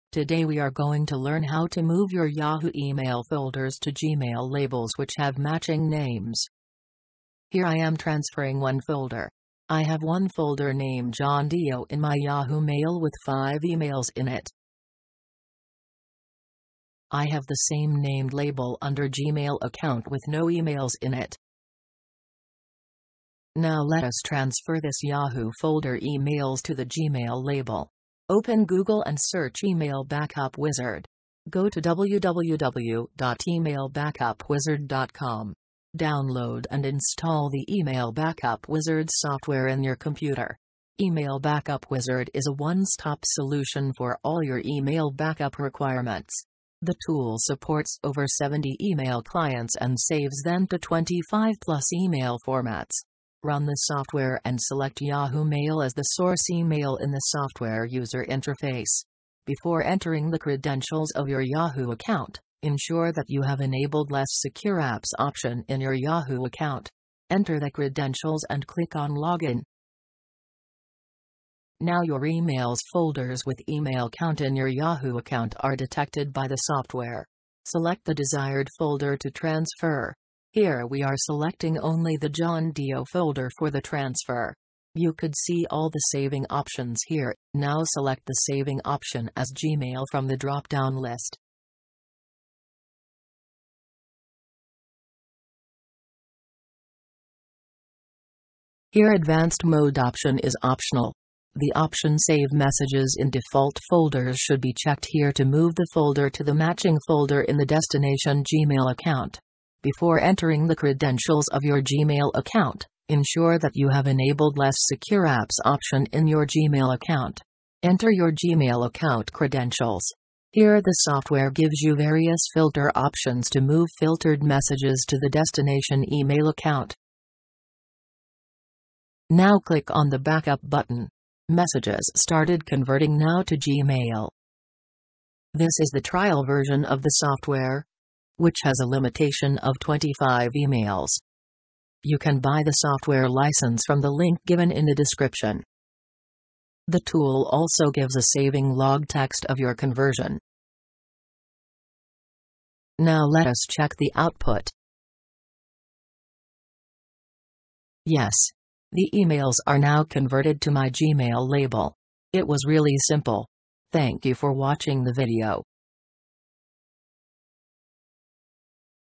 Move Yahoo Mail Folders to Gmail Labels - Transfer Emails with Attachments from Yahoo to Gmail
How to move your Yahoo Email Folders to Gmail? Here you will discover steps to transfer old emails from yahoo to gmail as well as learn how to transfer all emails from yahoo to gmail account.

There are two ways to transfer your Yahoo folders to Gmail. One method is transferring yahoo folders to under Gmail Labels which are created and separated from original folders, keeping the folder hierarchy.

Second method is moving Yahoo folders to Gmail folders/labels which are already existing with matching the folder names.

We have shown the second method of conversion in this video. Kindly watch the video to know how the process of conversion.

Note: You have to enable ‘Allow Less Secure Apps’ and IMAP options in your Yahoo and Gmail accounts for a successful conversion.

Watch our recently uploaded videos:
Office 365 to Gmail Conversion: @
Gmail to Office 365 Conversion: @
Hosted Exchange to Office 365 Conversion: @